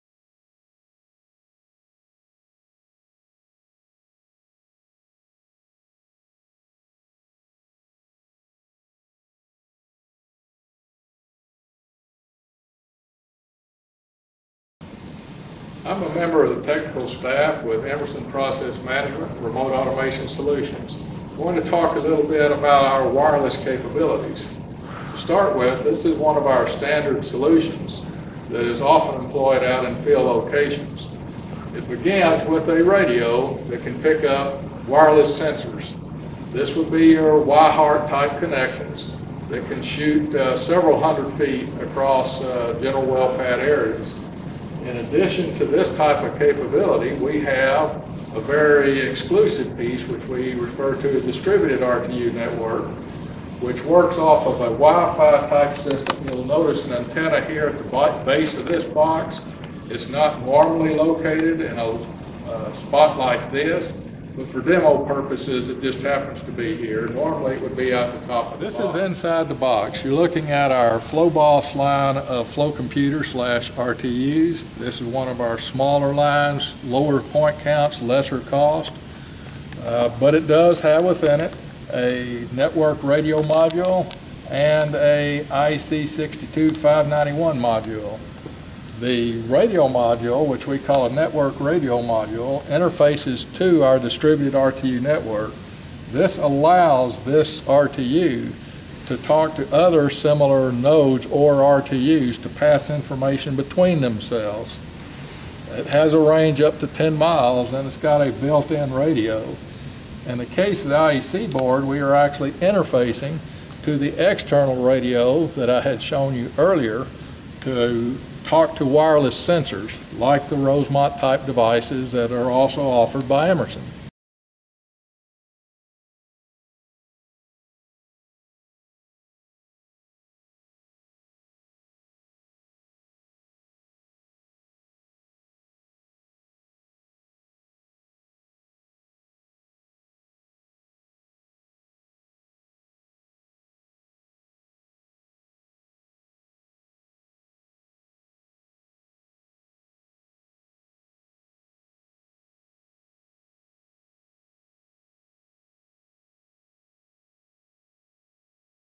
Distributed RTU Network
A Intro to Distributed RTU Network and how you can distribute wireless control across a network of RTU/Flow Computers.

Key Benefits:
- Reach First Production Faster
- Optimize Facility Development Costs
- Improve Operations Efficiency
- Maximize Lifetime Production & Yield